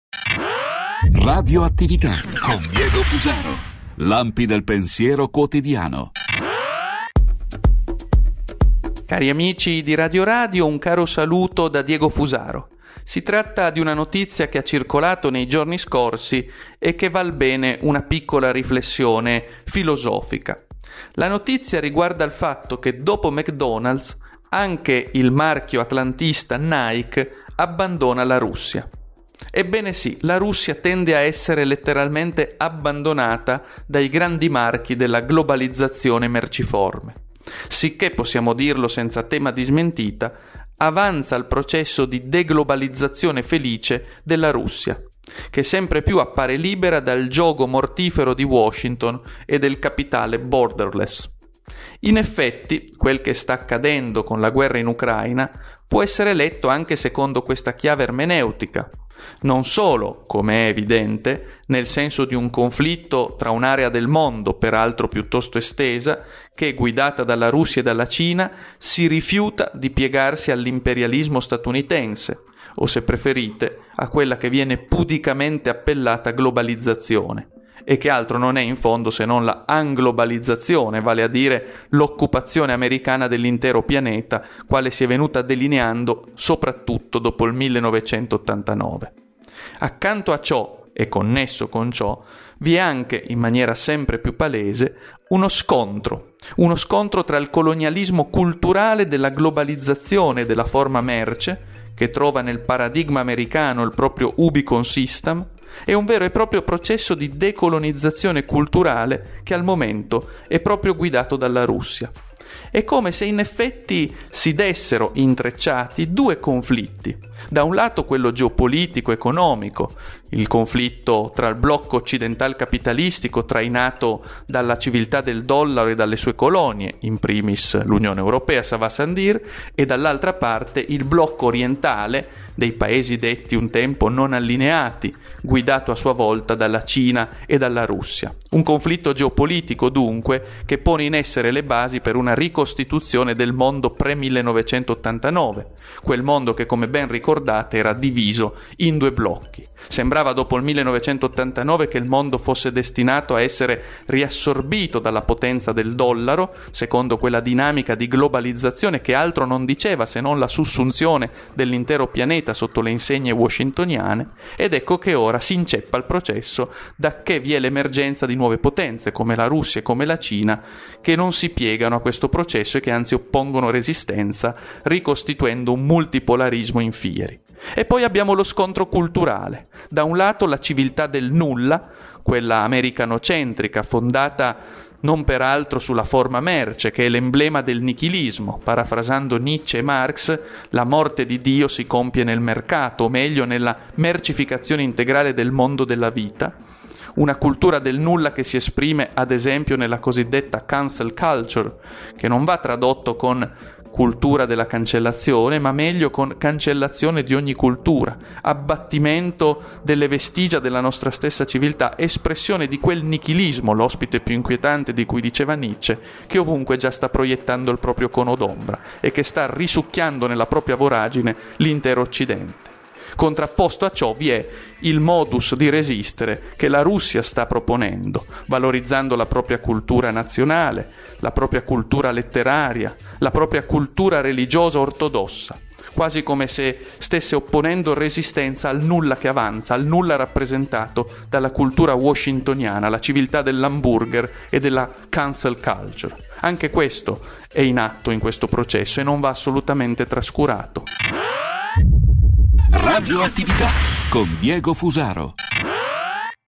Russia, ora anche Nike esce allo scoperto: il marchio atlantista "entra in guerra" con il Cremlino
Si tratta di una notizia che ha circolato nei giorni scorsi e che val bene una piccola riflessione filosofica: la notizia riguarda il fatto che dopo McDonald's, anche il marchio atlantista Nike abbandona la Russia. Ebbene sì, la Russia tende ad essere abbandonata dai grandi marchi della globalizzazione merciforme, sicché possiamo dirlo senza tema di smentita avanza il processo di deglobalizzazione felice della Russia che sempre più appare libera dal giogo mortifero di Washington e dal capitale Borderlands. In effetti, quello che sta accadendo con la guerra in Ucraina può essere letto anche secondo questa chiave ermeneutica: non solo (com'è evidente) come il senso di un conflitto tra un area del mondo, per altro piuttosto estesa che, guidata dalla Russia e dalla Cina , si rifiuta di piegarsi all'imperialismo statunitense o (se preferite) a quella che impunemente viene chiamata globalizzazione e che altro non è infondo se non 'anglobalizzazione': l'occupazione americana dell'intero pianeta quale si è venuta delineando soprattutto dopo il 1989.

Accanto a ciò e connesso con ciò, vi è anche in maniera più palese uno scontro tra il colonialismo culturale della globalizzazione della forma merce che trova nel paradigma americano il proprio ubi consistam e un vero e proprio processo di decolonizzazione culturale guidato al momento dalla Russia. È proprio come se si dessero intrecciati due conflitti: da un lato quello geopolitico ed economico, il conflitto tra il blocco occidental-capitalistico, trainato dalla civiltà del dollaro e dalle sue colonie (in primis l'Unione Europea) e dall'altra parte il blocco orientale dei paesi detti un tempo 'non allineati', guidato a sua volta dalla Cina e dalla Russia. Un conflitto geopolitico dunque, che pone in essere le basi per una ricostituzione del mondo pre 1989 che ricordiamo era diviso in due blocchi. Sembrava dopo il 1989 che il mondo fosse destinato a essere riassorbito dalla potenza del dollaro secondo quella dinamica di globalizzazione che altro non diceva se non la sussunzione dell'intero pianeta sotto le insegne washingtoniane. Ed ecco che ora s'inceppa il processo dacché vi è l'emergenza di nuove potenze come la Russia o la Cina che non si piegano a questo processo e che anzi oppongono resistenza ricostituendo un multipolarismo in fieri.

CONTINUA SU: ...

RadioAttività - Lampi del pensiero quotidiano con DiegoFusaro